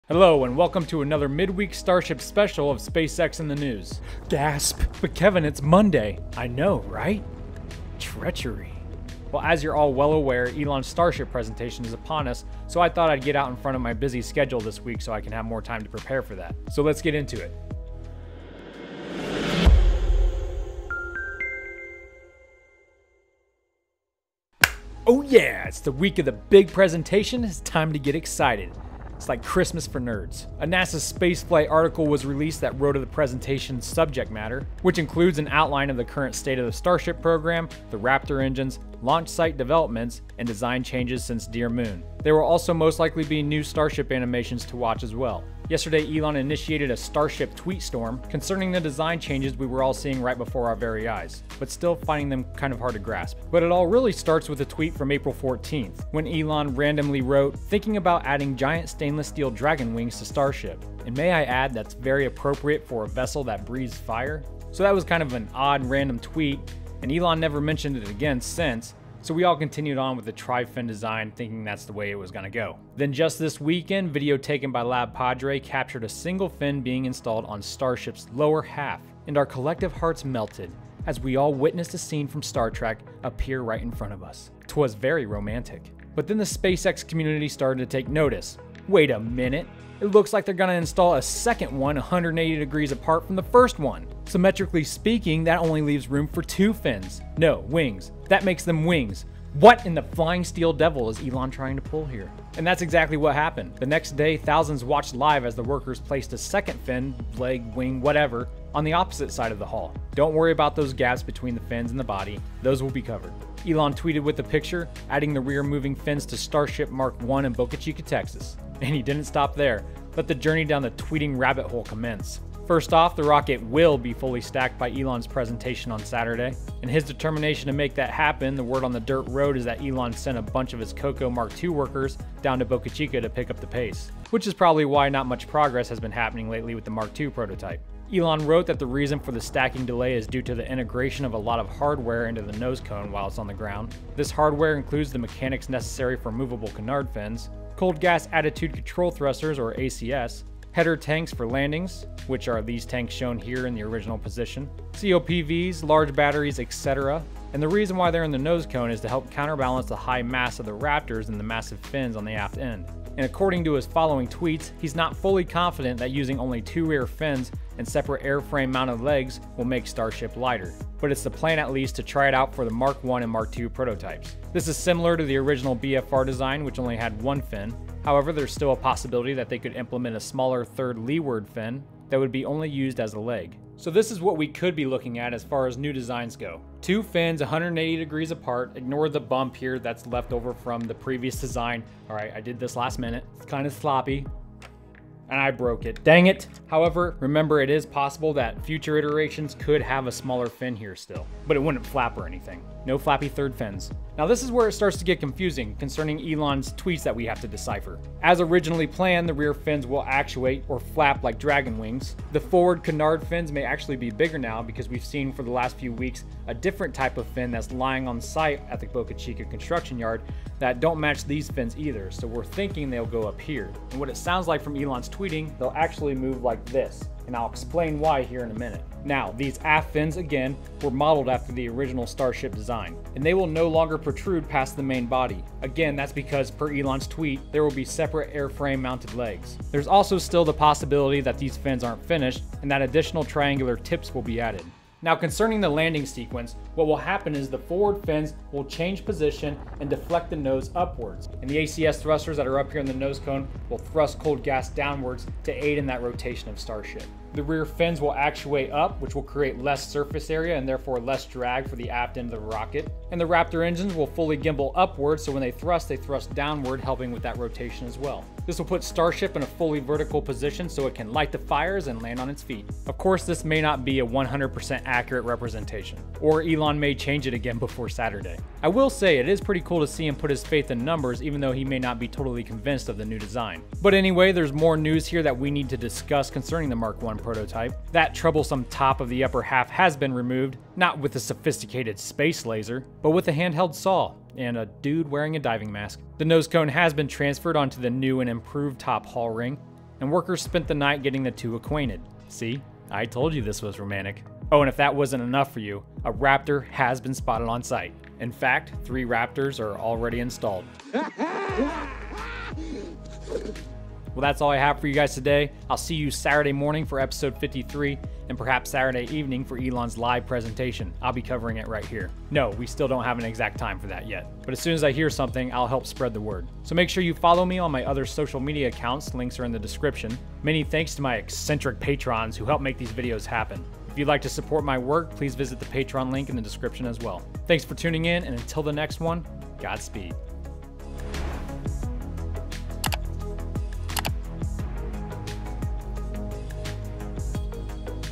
SpaceX Starship Gets Its Wings - New Design Revealed | SpaceX in the News
SpaceX Starship midweek episode #61 of SpaceX in the News - We learn about all the latest developments concerning Elon Musk's Mars rocket prototype.

Executive Producers: The Kreators

Thumbnail image: Original by Brandon Bolleber

Main Sources:
Maria Pointer